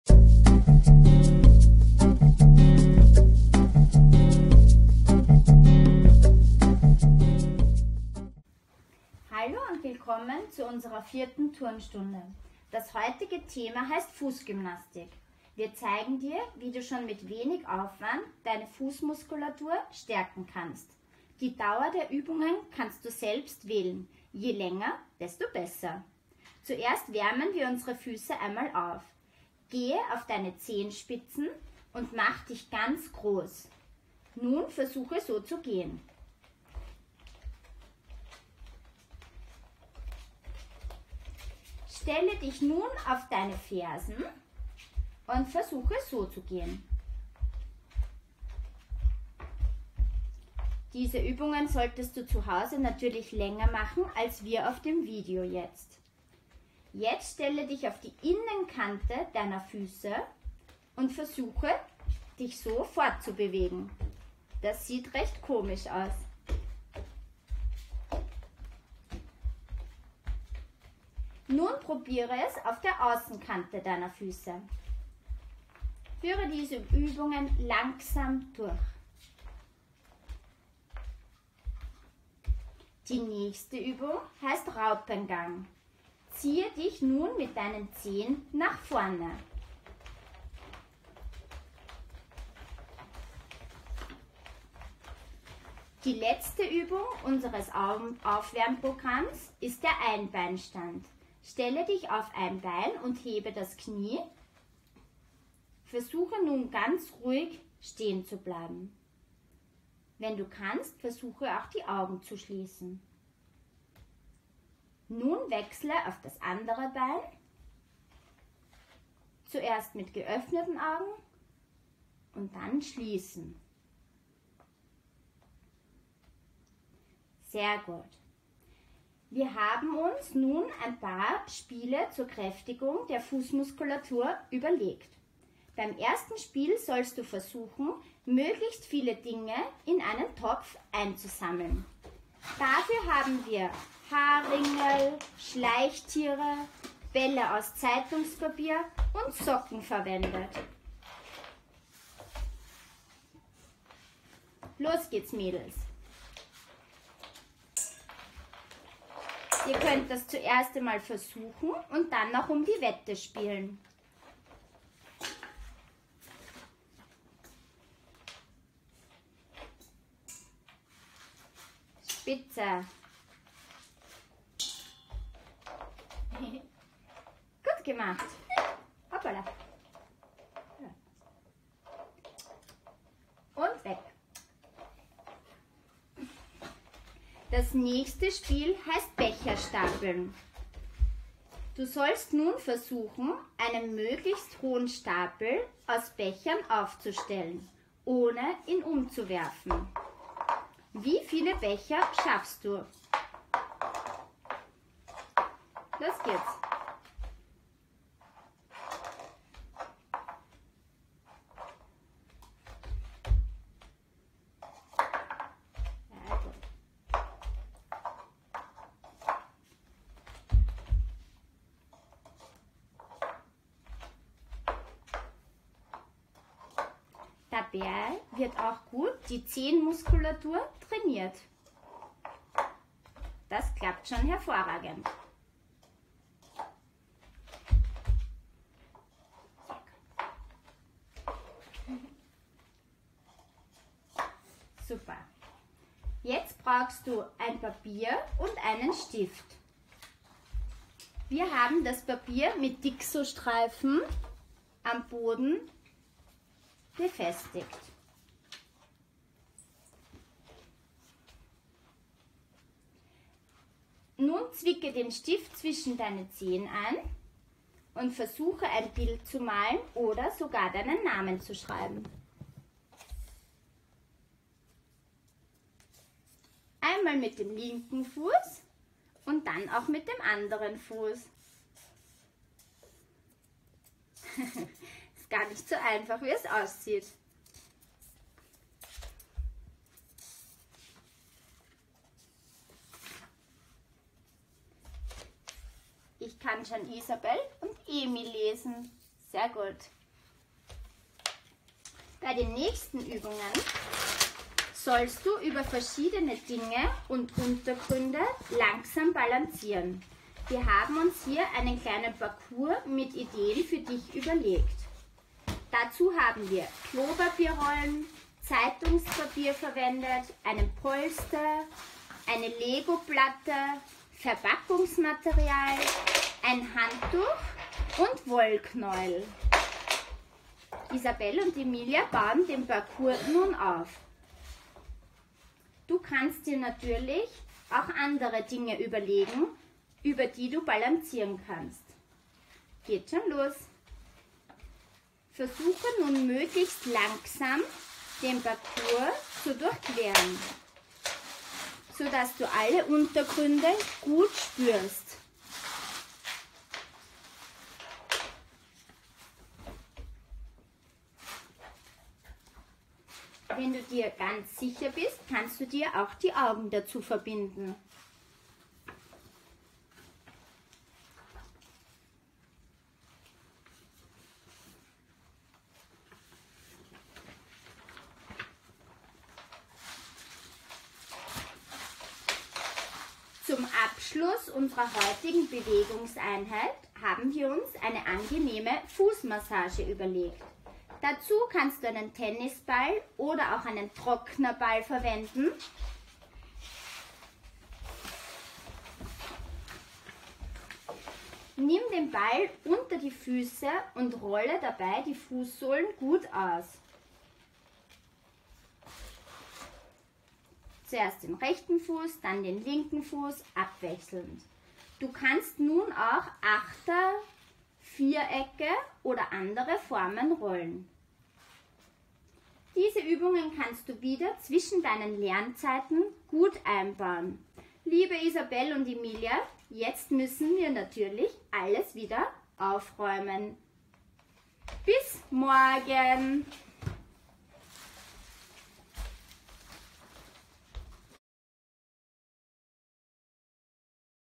Onlinebewegungseinheit #31: Bewegung@Home für die VS - Fußmuskulatur
Die tägliche Turnstunde für die Volksschule kommt nachhause. Kurze Bewegungsvideos für Primarstufenschüler/innen – einfach zuhause nachmachen!
Eine Kooperation der VS Pernitz und der PH NÖ.